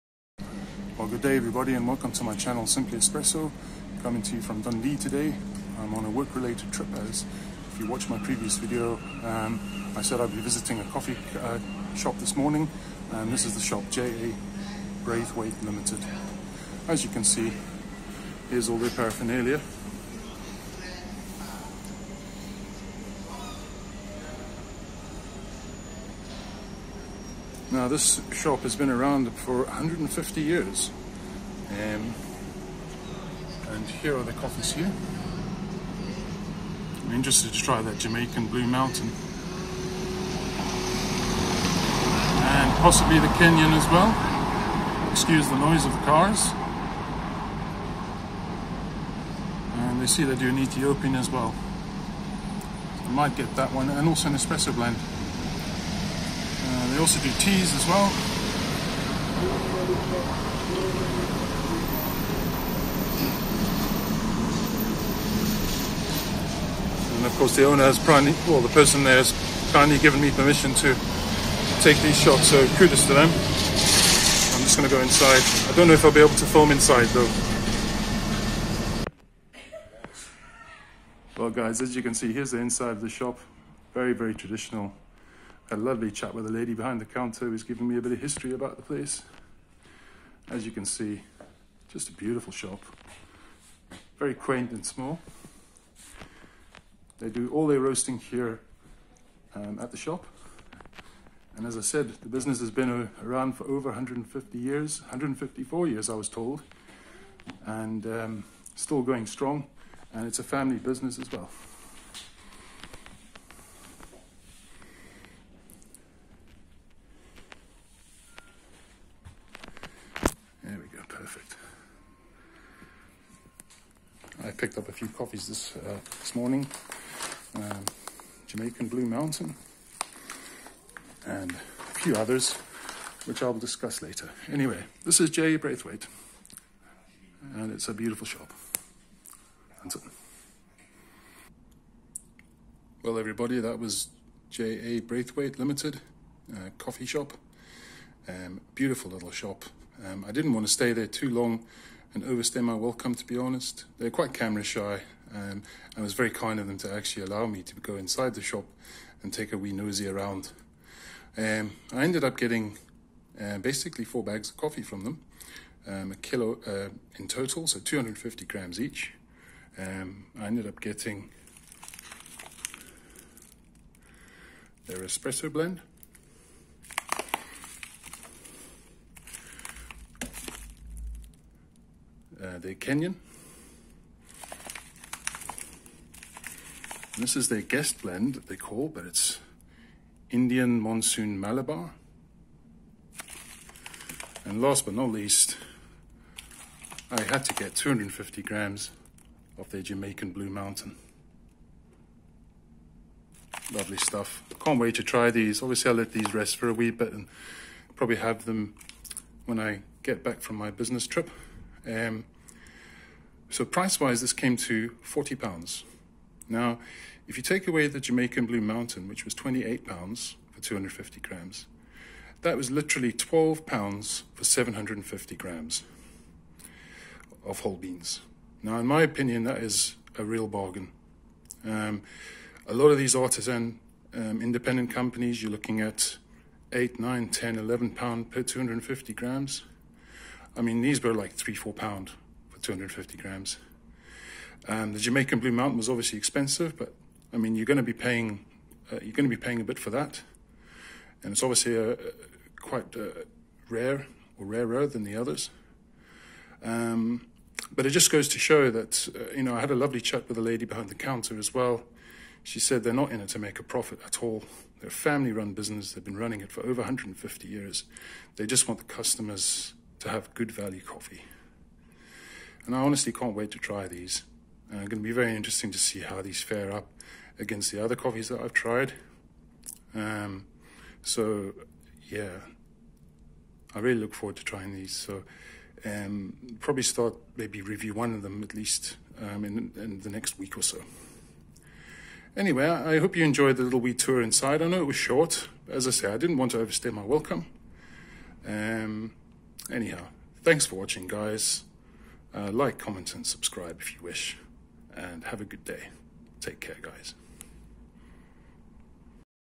154 Year Old Coffee Merchant JA Braithwaite Inside Tour & Coffee Haul Jamaican Blue Mountain
A wee look inside the beautiful and traditional coffee roaster J A Braithwaite and coffee haul from them